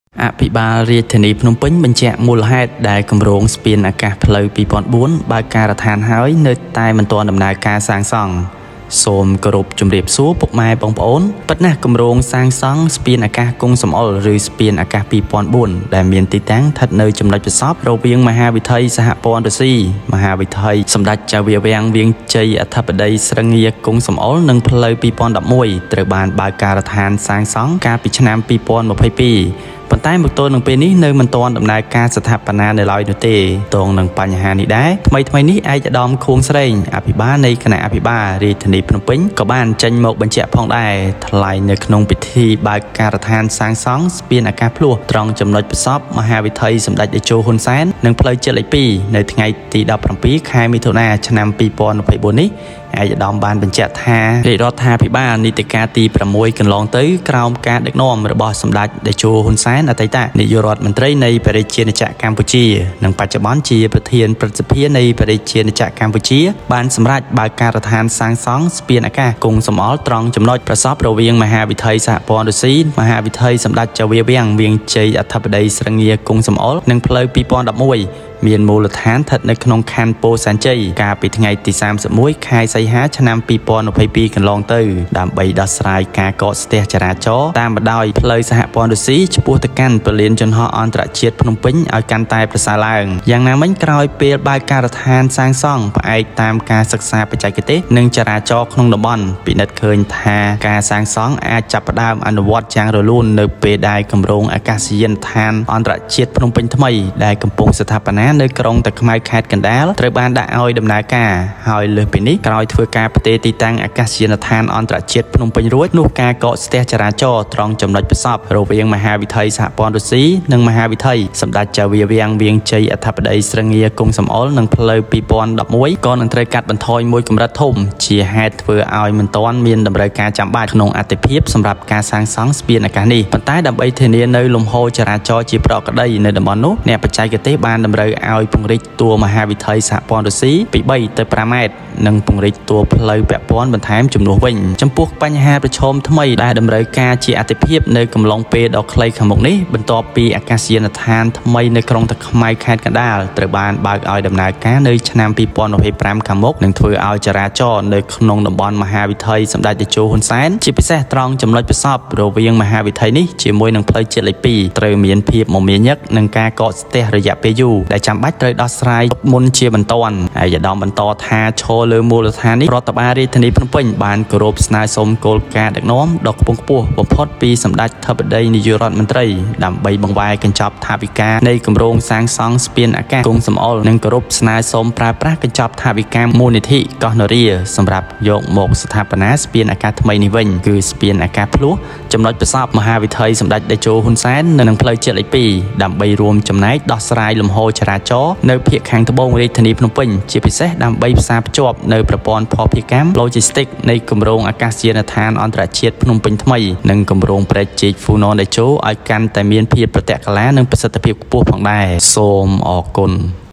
មកដឹងមូលហេតុដែល គម្រោងស្ពានអាកាសផ្លូវ២០០៤ បើកការដ្ឋានហើយ តែមិនទាន់ដំណើរការសាងសង់
Indeed, the Kong Sam Ol flyover project, or 2004 flyover, located at the intersection of Russian Boulevard, Samdech Chao Feang Boulevard, Vireak Chey Boulevard, President Srengkar Kong Sam Ol, and 2011 was opened in 2022. But so far the construction is not yet in progress. In relation to this issue, recently, His Excellency Khuong Sreng, Governor of the Board of Governors of Phnom Penh also issued a statement.
Speaking at the groundbreaking ceremony for the construction of the twin flyovers at the intersection of Samdech Hun Sen Blvd. and National Road No. 2 on June 17, 2024, HE confirmed the Royal Government of the 6th legislature under the leadership of Samdech Techo Hun Sen Former The Prime Minister of the Kingdom of Cambodia and the current President of the Senate of the Kingdom of Cambodia has decided to open the construction site of the Kong Sam Ol flyover at the intersection of Russian Federation Blvd. Por Sen Chey on August 31, 2022 to solve traffic congestion along the Russian Federation Road to Phnom Penh International Airport.
After opening the site based on the technical study and traffic in the area, it can be seen that the construction can start smoothly when the new international airport project under construction in Takhmao City, Kandal Province has been launched and after this transfer At Phnom Penh International Airport, traffic congestion at the intersection of Russian Blvd. and Boulevard Boulevard will be reduced to a large extent, making it unnecessary to prioritize. Build this flyover. However, in order to ensure normal traffic flow in the area, technicians were required to widen the Russian Federation Boulevard from 3 meters to 5 meters and widen the relevant lanes instead.
The new challenges that will be a priority in the near future after the new airport in Takhmao City, Kandal Province will open in 2025 will increase traffic in the area of ​​Samdech Hun Sen Blvd. At the intersection of this boulevard and National Road 2, there is a lot of traffic and long-term congestion that needs to be addressed in advance.
His Excellency continued that on this basis, the Phnom Penh Capital Administration has requested the highest leadership from the Prime Minister to divert the budget for the construction of the Kong Sam Ol Bridge and to request the use of the Koh Nora Fund for the construction of the new flyover. Twin flyovers at the intersection of Samdech Hun Sen Blvd. and National Road 2) to contribute to the smooth flow of traffic in the south of Phnom Penh, especially to better integrate the logistics system of the new International Airport and Prek Chik Funan Techo projects. High cell interface and efficiency.

-ក្របខណ្ឌ និងការប្រលងក្រសួងសាធារណការ និងដឹកជញ្ជូន Ministry of public works and Transport